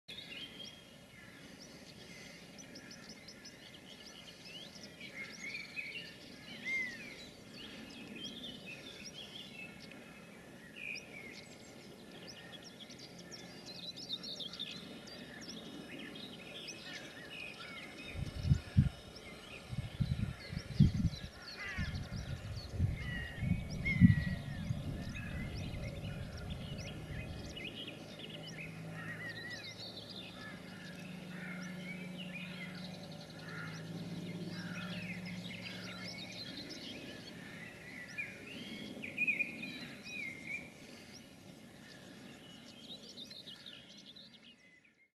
Torenvalk valt Buizerd aan
Torenvalk valt Buizerd aan, gefilmd in de Kaaistoep Tilburg, 30 mei 2017.